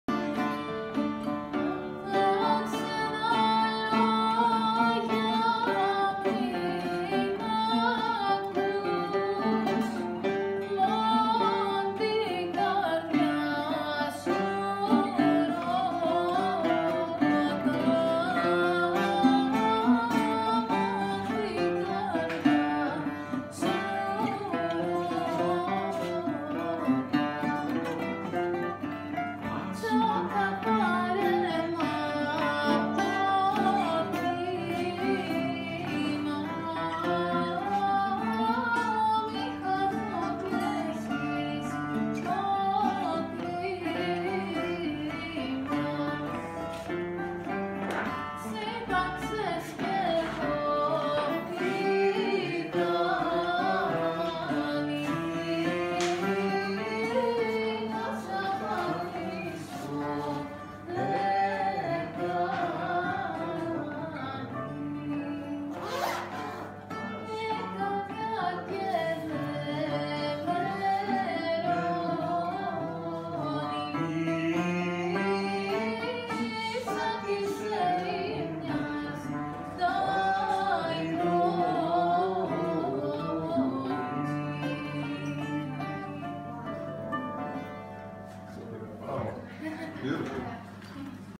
Rebetiko Jam Nights Yiamas Brunswick 7.11.18
Stella Aslanoglou- Vocals/Oud
Con Kalamaras - Bouzouki
Wayne Simmons -Guitar
Spiro Bouzouki